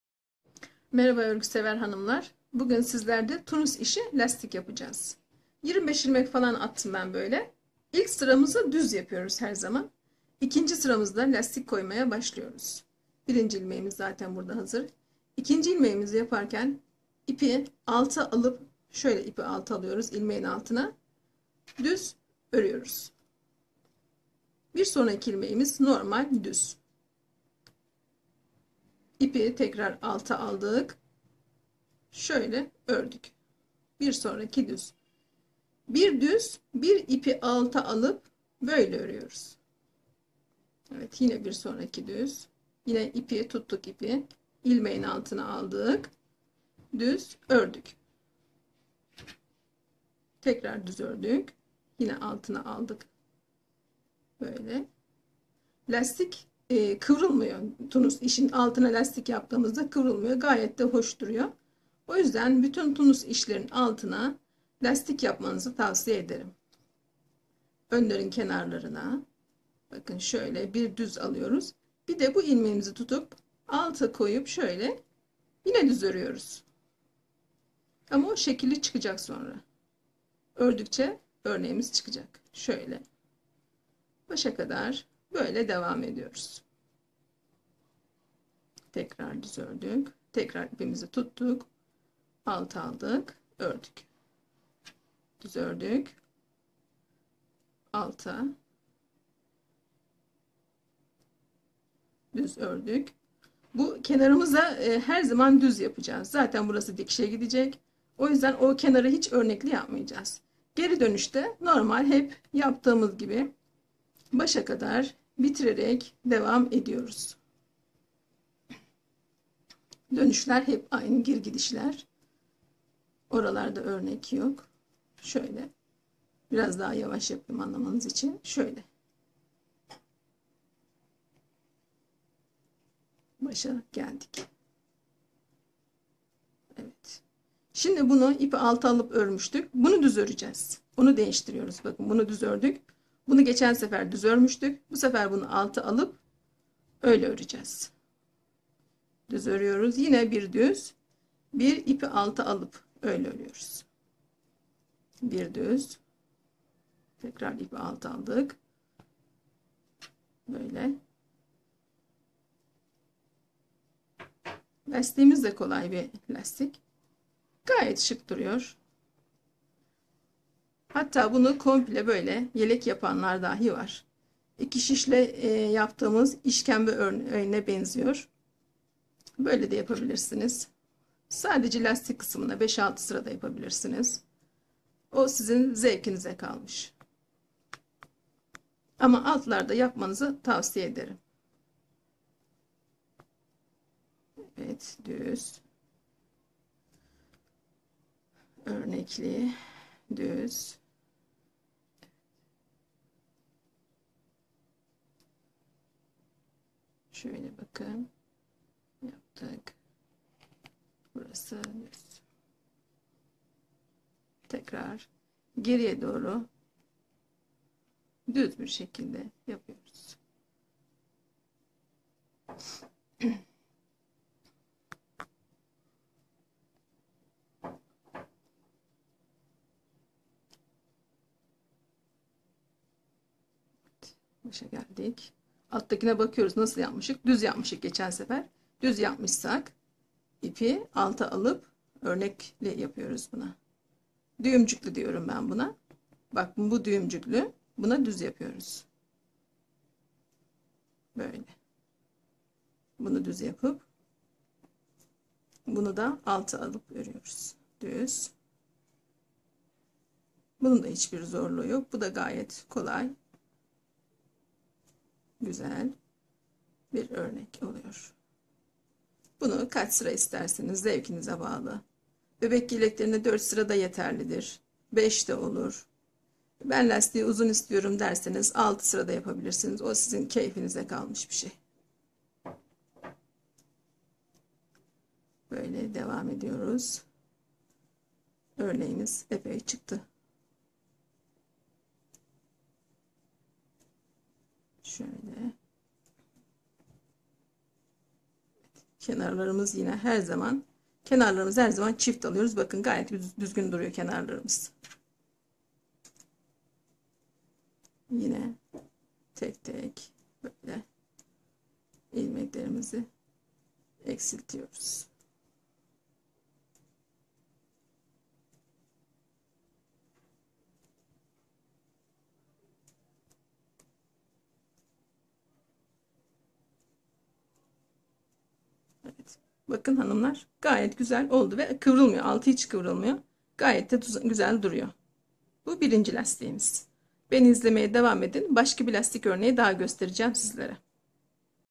Tunus işi lastik yapımı anlatımlı
Tunus işi lastik nasıl yapılır videomuzu izleyerek öğrenebilirsiniz.